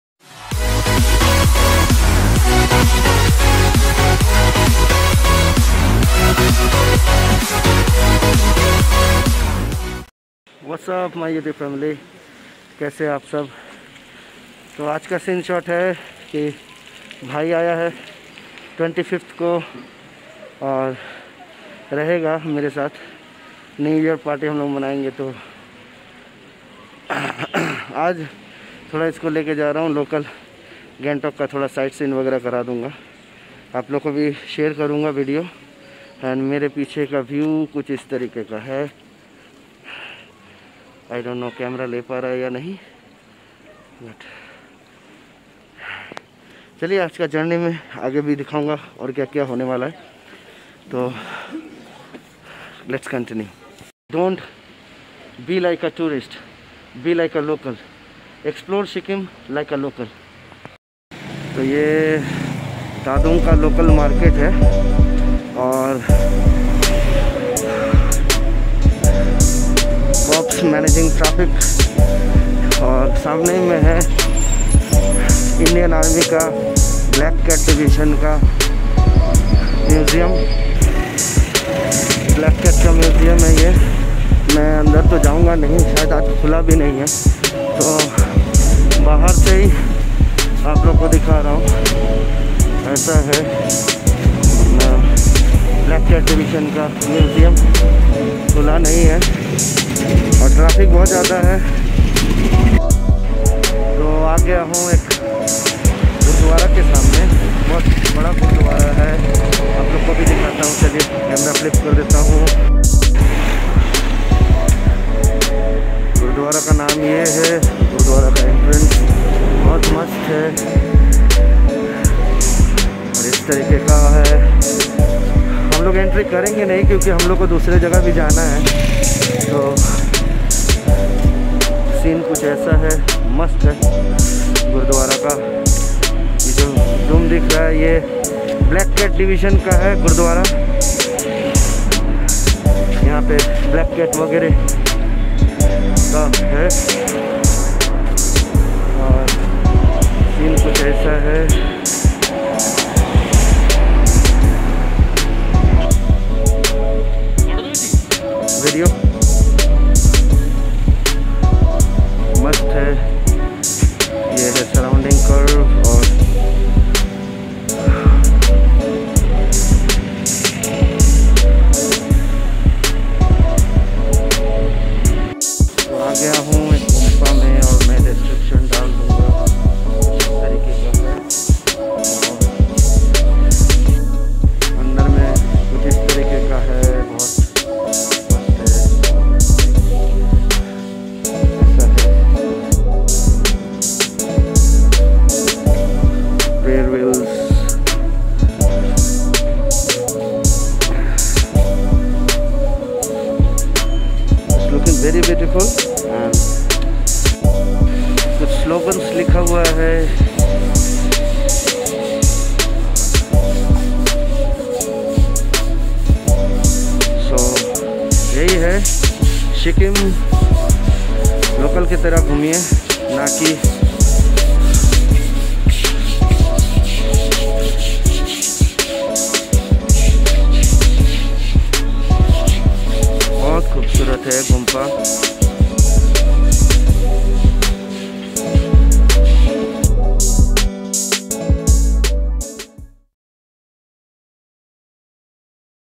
Tadong sikkim 🐲 Ep:-5
I'm in Tadong Sikkim right now ... Today we're going for local walking and we will share beautiful local activity surroundings and it's nature hope u PPL gnna love ep :-4 of my vlog comment me below 👇 about the vlog and what should my NXT vlog will be...join me in the journey of sikkim ... explore Sikkim like local 😍❤️